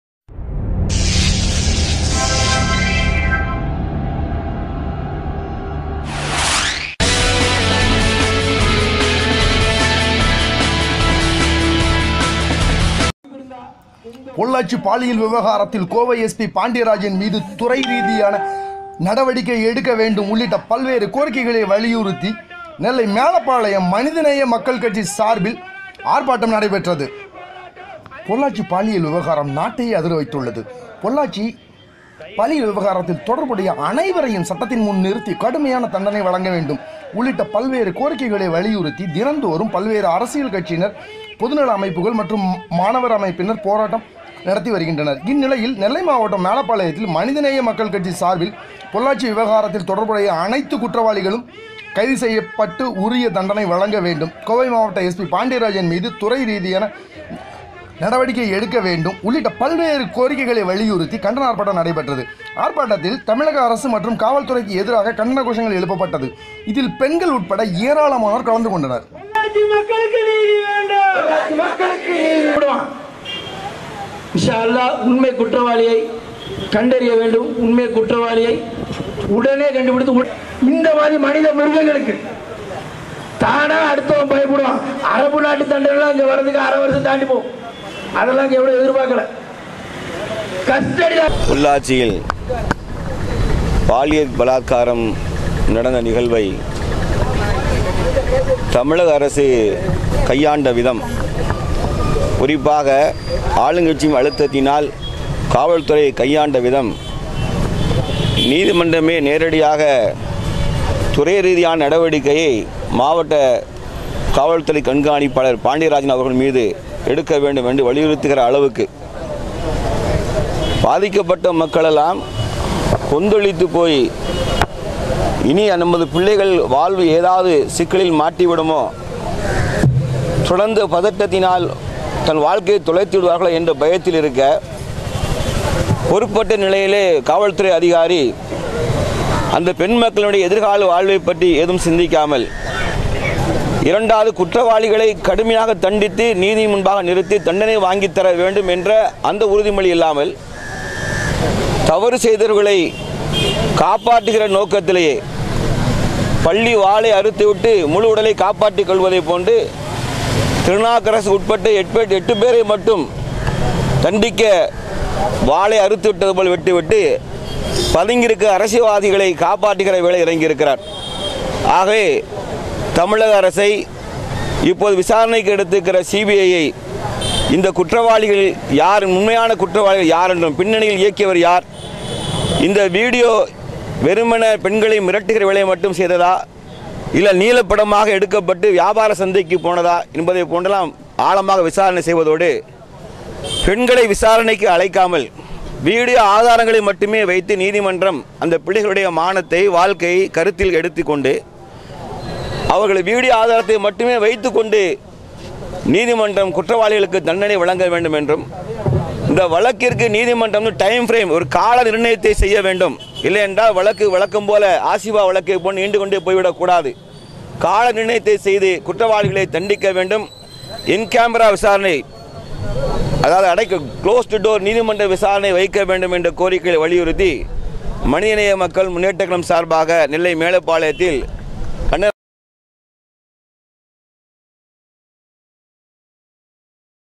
எஸ் பி மீது துறை ரீதியான நடவடிக்கைமனிதநேய மக்கள் கட்சி
பொள்ளாச்சி பாலியல் விவகாரத்தில் கோவை எஸ் பி பாண்டியராஜன் மீது துறை ரீதியான நடவடிக்கை எடுக்க வேண்டும் உள்ளிட்ட பல்வேறு கோரிக்கைகளை வலியுறுத்தி நெல்லை மேலப்பாளையத்தில் மனிதநேய மக்கள் கட்சி சார்பில் கட்சியினர் ஆர்ப்பாட்டம்

பொள்ளாச்சி பாலியல் விவகாரம் நாட்டையே அதிர வைத்துள்ளது பொள்ளாச்சி பாலியல் விவகாரத்தில் தொடர்புடைய அனைவரையும் சட்டத்தின் முன் நிறுத்தி கடுமையான தண்டணை  வழங்க வேண்டும் உள்ளிட்ட பல்வேறு கோரிக்கைகளை வலியுறுத்தி தினந்தோறும் பல்வேறு அரசியல் கட்சியினர் பொதுநல அமைப்புகள் மற்றும் மாணவர் அமைப்பினர் போராட்டம் நடத்துகின்றனர் இந்நிலையில் நெல்லை மாவட்டம் மேலப்பாளையத்தில் மனிதநேய மக்கள் கட்சி சார்பில் பொள்ளாச்சி பாலியல் விவகாரத்தில் தொடர்புடைய அனைத்து குற்றவாளிகளும் கைது செய்து உரிய தண்டனை வழங்க வேண்டும், கோவை மாவட்ட எஸ்பி பாண்டியராஜன் மீது துறை ரீதியான நடவடிக்கை எடுக்க வேண்டும் உள்ளிட்ட பல்வேறு கோரிக்கைகளை வலியுறுத்தி கண்டன ஆர்ப்பாட்டம்  நடைபெற்றது. ஆர்ப்பாட்டத்தில் தமிழக அரசு மற்றும் காவல் துறைக்கு எதிராக கண்டன கோஷங்கள் எழுப்பப்பட்டது. இதில் பெண்கள் உள்பட திரளானோர் கலந்து கொண்டனர்